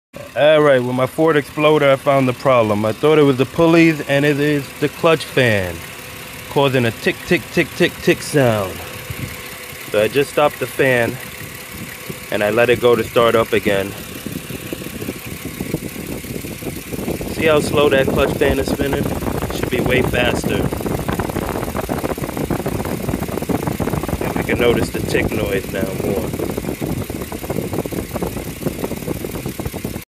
Tick Tick Tick 💣 It was just the clutch fan 🤠
This 2010 V6 Ford Explorer Is Starting To Make Me Go MAD 😡 I've changed A Couple Of Pulleys & I'm Still Getting This Ticking Noise When Starting The Car In The Morning. Figured Out That It's The Clutch Fan Making Noise 🤠